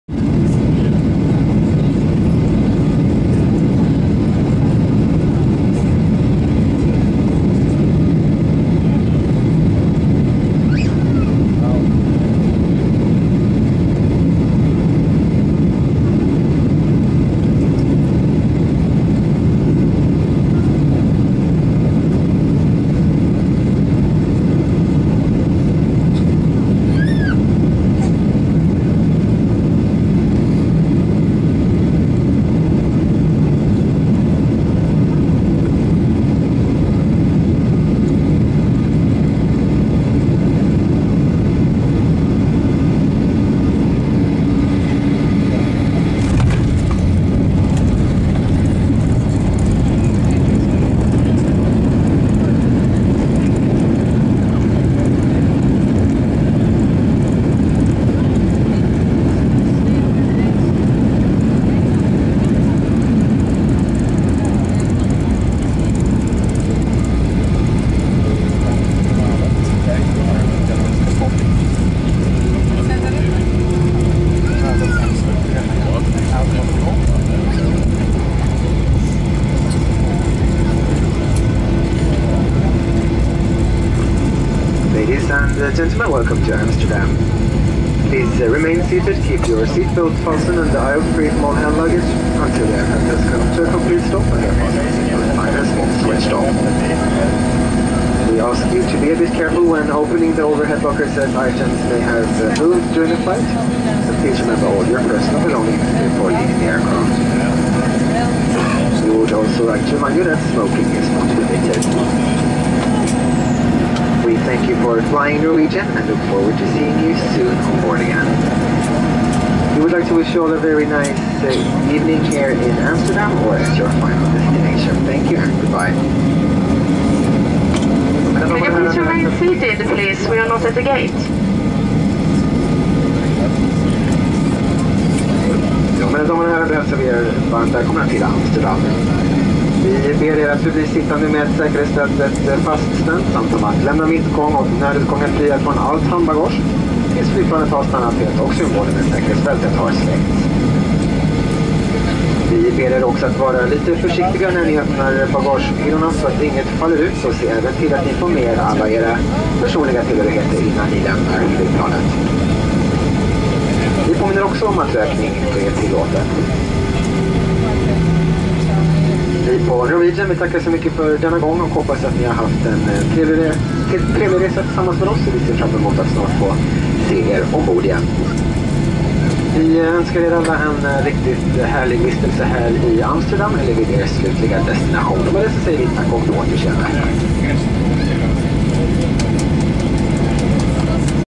Norwegian Air Shuttle, 737-800, landing in Amsterdam (HD)
Norwegian Air Shuttle flight number 4531 landing in Amsterdam, Netherlands, arriving from Stockholm, Sweden (Show more for flight info and flight review)

Date: 7/17/14
Tail Number: LN-DYK (named Carl Larsson)
Seat Number: 19F

-Must give credit to the Norwegian staff first of all. We were scheduled to depart at 6:30 pm (18:30). The aircraft arrived at 6 pm (18:00). Yet, the entire aircraft was deplaned, cleaned, boarded, and pushed back by 6:40 pm (18:40)!!! Despite the chaotic boarding, boarding only took about 15-20 minutes as Norwegian used a jetway as well as stairs at the back of the aircraft. The crew was nice, flight was fast, and that's all there was! No customs in AMS as Sweden and The Netherlands are both EU countries!